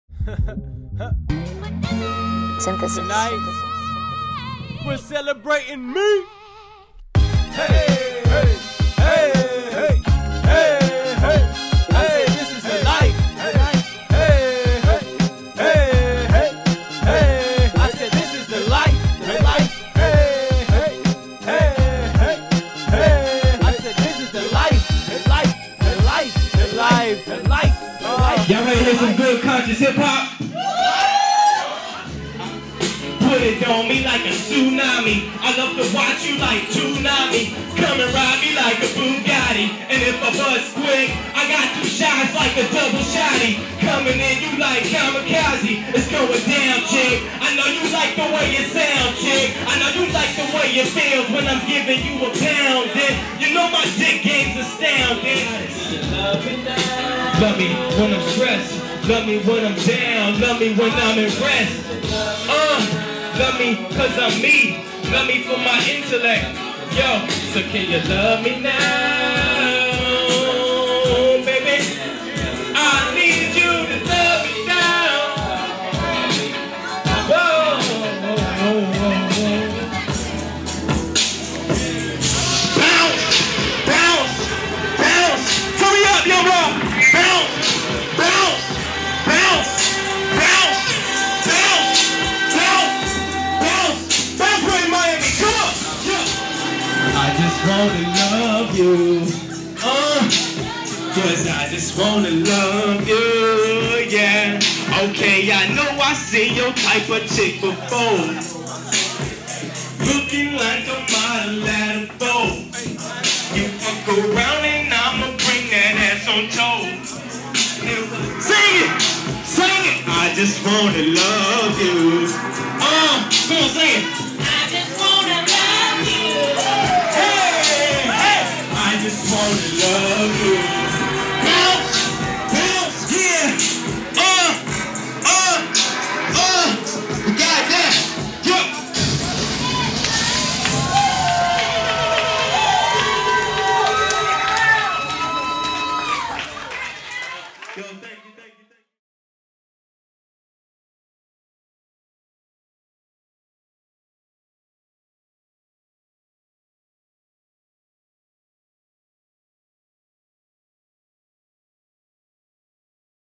Jay One @ The Hub Sobe
Jay One performing at a club on south beach